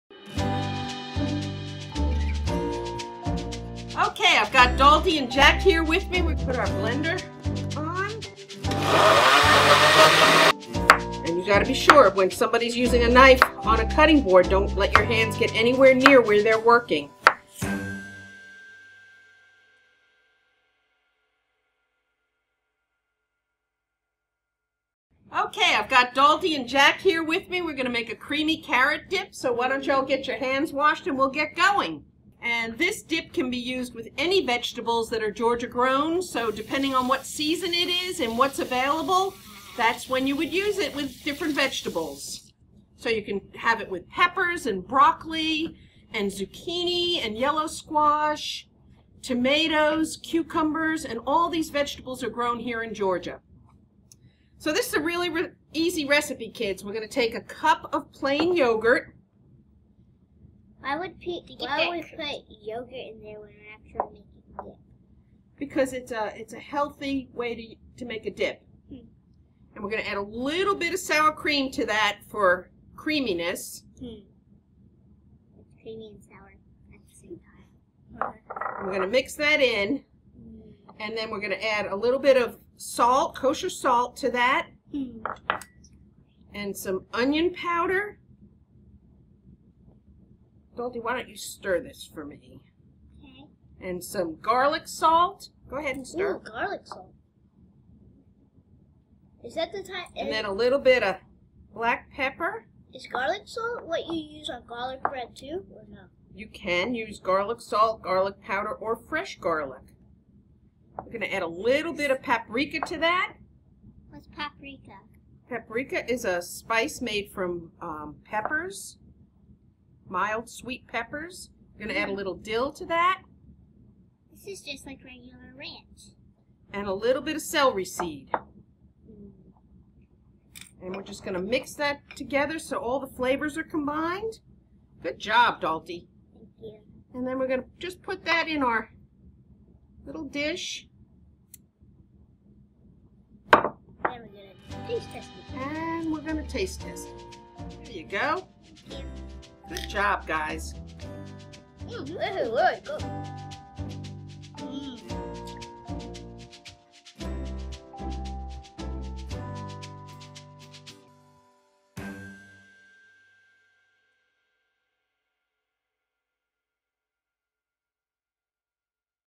Creamy Carrot Dip
Join Jack and Dalty as Chef Holly teaches them how to make a delicious Creamy Carrot Dip.  This yogurt-based dip is simple to make and tastes wonderful with Georgia Grown carrots and other vegetables!